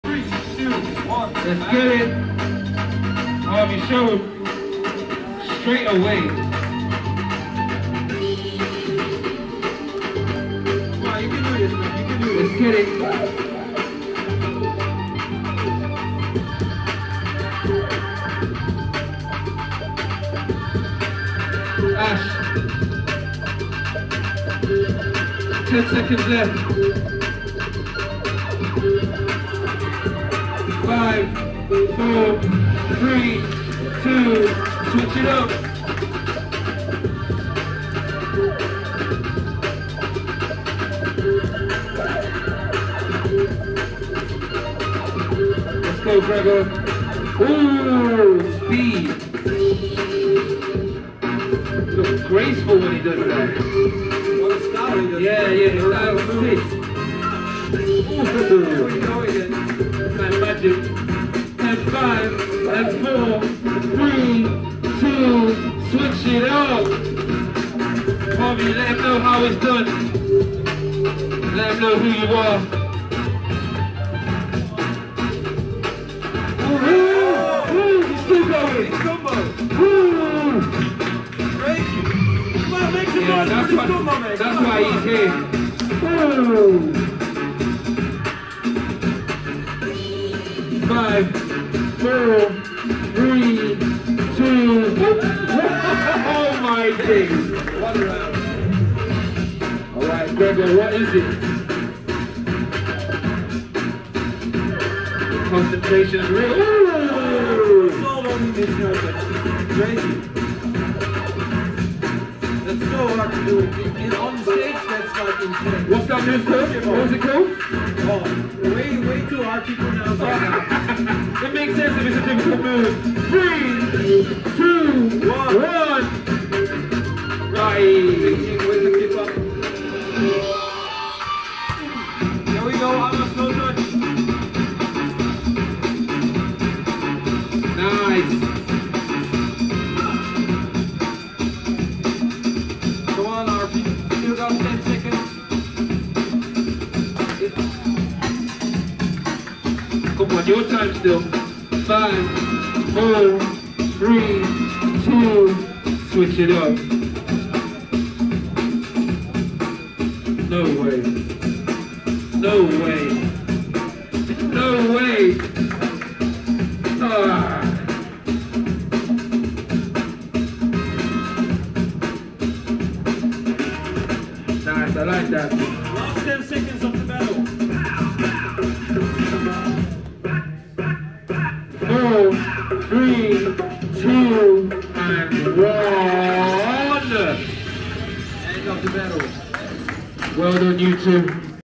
Gregor vs Harvey - Top 16 | Ultimate Freestyle 2023
Gregor Russell vs Harvey Cameron - Top 16 | UK Freestyle Football Championship 2023 [Ultimate Freestyle]

Winner: Gregor

Partners: @ultimatefreestylecomp
Freestylers: @gregorfreestyler, @harveycameronfs
Thumbnail: @romanbrezovsky